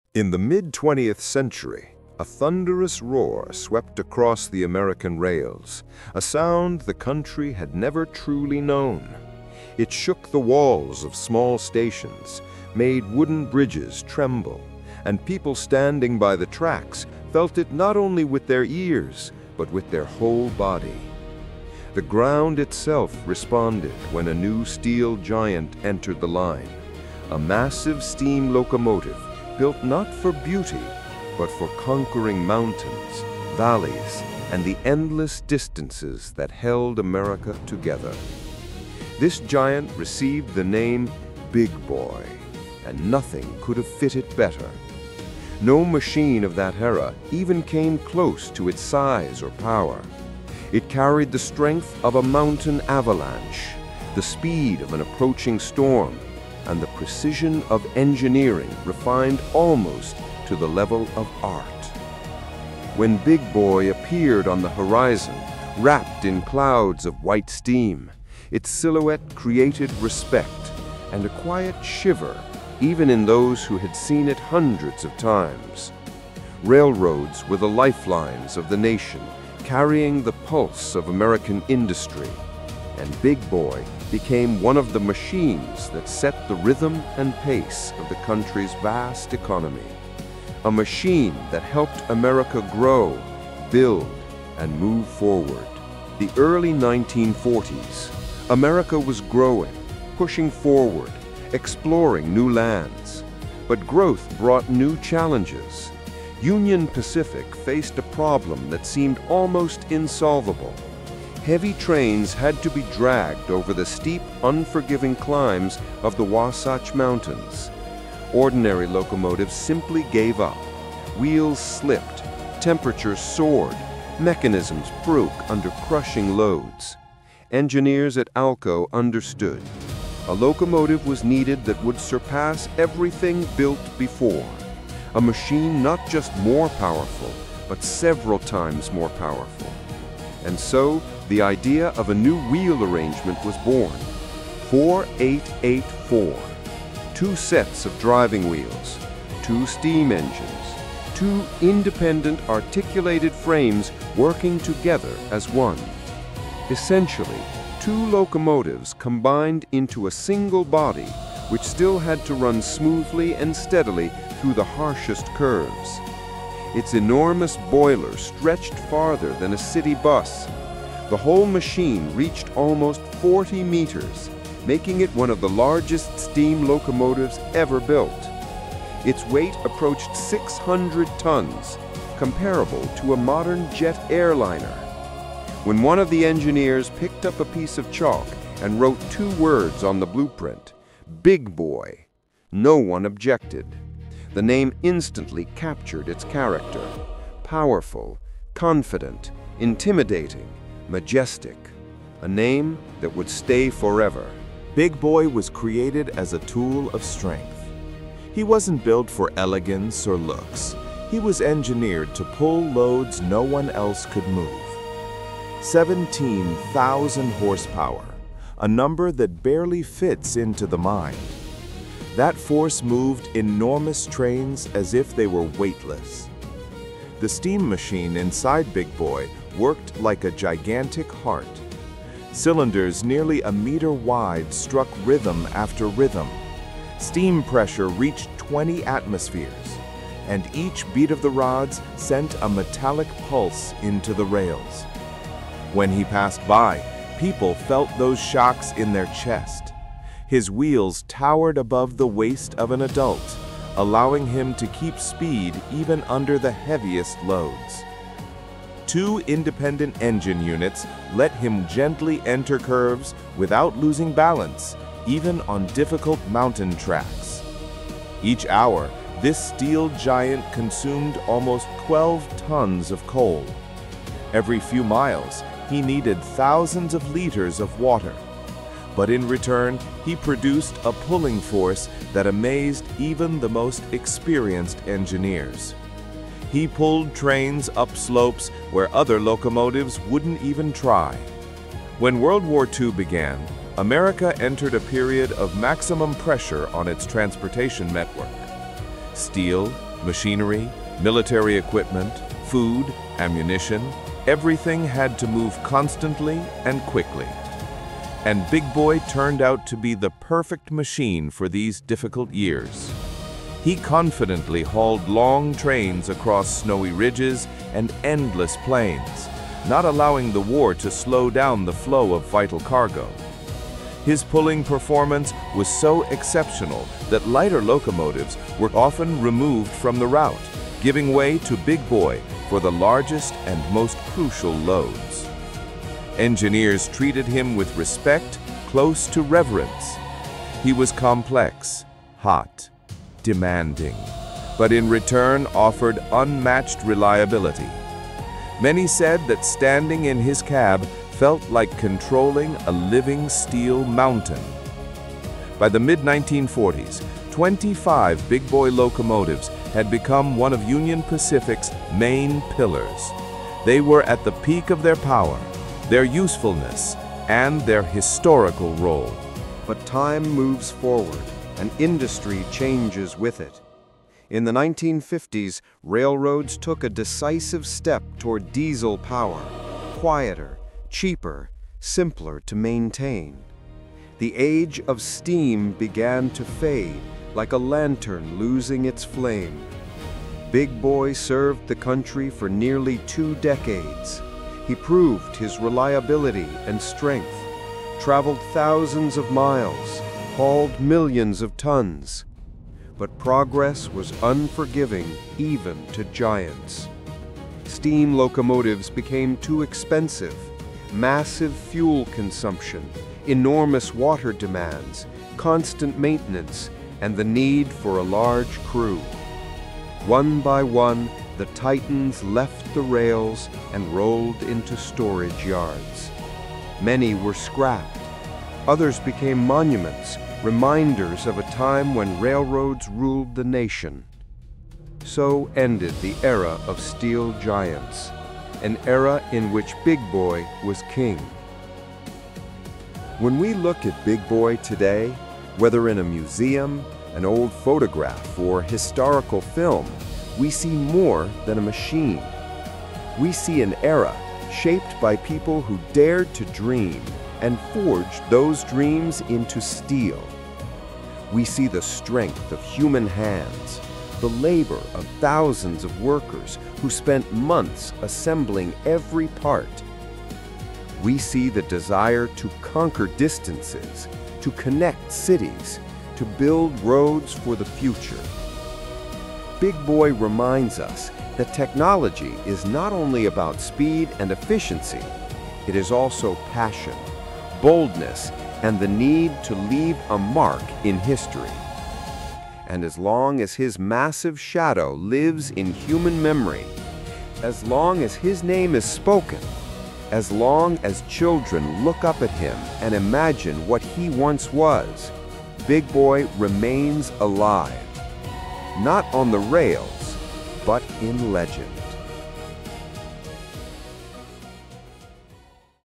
Big Boy | You Won’t Believe What This Monster Could Do
Big Boy wasn’t just a train — he was a mechanical monster built to conquer mountains, brutal weather, and the heaviest freight America had ever moved.
In this documentary, you’ll discover what this giant was truly capable of: unbelievable horsepower, extreme engineering choices, record-breaking performance, and the real story behind why steam titans like Big Boy disappeared from the rails.
We explore the full evolution of the Union Pacific Big Boy — from its design and construction to its legendary trips across the Wasatch Range. You’ll see how this 4014 locomotive became the symbol of American railroads, why engineers considered it the peak of steam locomotive engineering, and how it compared to other historical engines across the world.
This video dives into the raw power of Big Boy 4014, the secrets behind its articulated frame, massive 4-8-8-4 wheel arrangement, and the engineering breakthroughs that made it the largest steam locomotive ever built.
If you love train documentaries, railroad history, steam engines, vintage locomotives, or American engineering, this is for you.
Big Boy wasn’t just big — he was something the world had never seen before.
Get ready — Big Boy’s real potential will shock you.